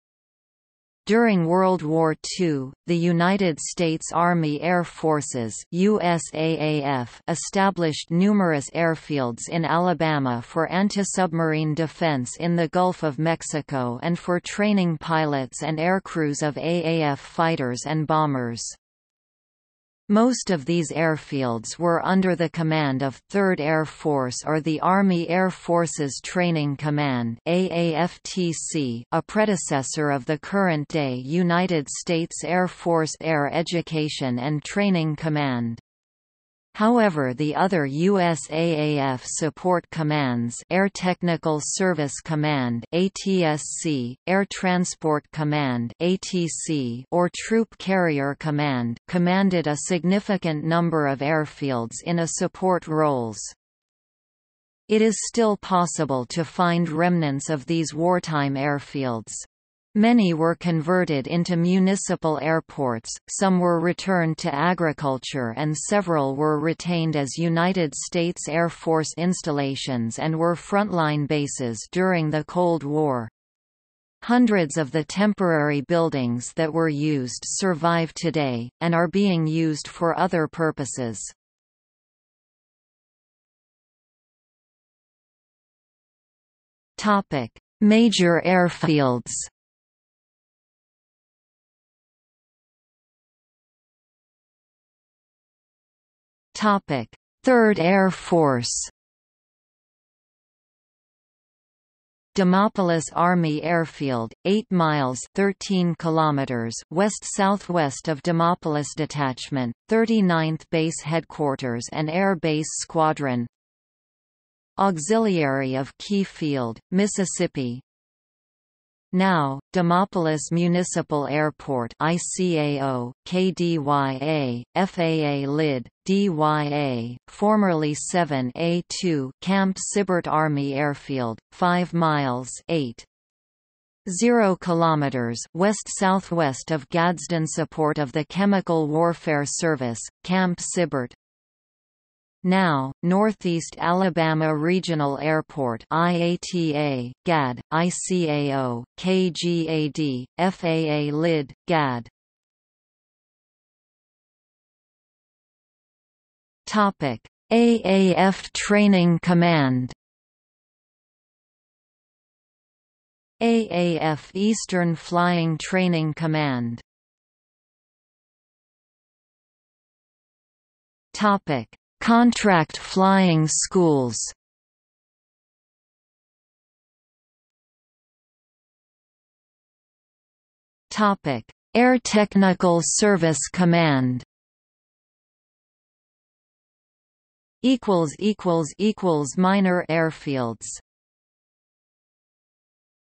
Alabama World War II Army Airfields | Wikipedia audio article
00:01:23 1 Major Airfields
00:01:33 1.1 Third Air Force
00:02:33 1.2 AAF Training Command
00:02:48 1.2.1 Contract Flying Schools
00:02:57 1.3 Air Technical Service Command

- Socrates

SUMMARY
During World War II, the United States Army Air Forces (USAAF) established numerous airfields in Alabama for antisubmarine defense in the Gulf of Mexico and for training pilots and aircrews of AAF fighters and bombers.
Most of these airfields were under the command of Third Air Force or the Army Air Forces Training Command (AAFTC) (A predecessor of the current-day United States Air Force Air Education and Training Command). However the other USAAF support commands (Air Technical Service Command (ATSC); Air Transport Command (ATC) or Troop Carrier Command) commanded a significant number of airfields in a support roles.
It is still possible to find remnants of these wartime airfields. Many were converted into municipal airports, some were returned to agriculture and several were retained as United States Air Force installations and were front-line bases during the Cold War. Hundreds of the temporary buildings that were used survive today, and are being used for other purposes.